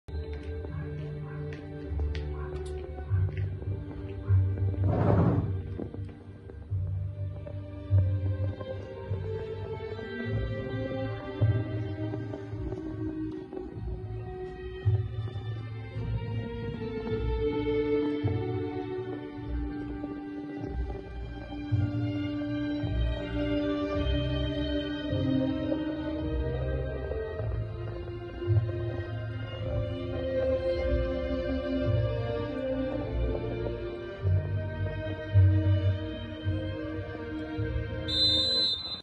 Minuto de silencio en Los Arcos en recuerdo a Diego Armando Maradona
Minuto de silencio en Los Arcos en recuerdo a Diego Armando Maradona previo al partido contra la Peña Deportiva.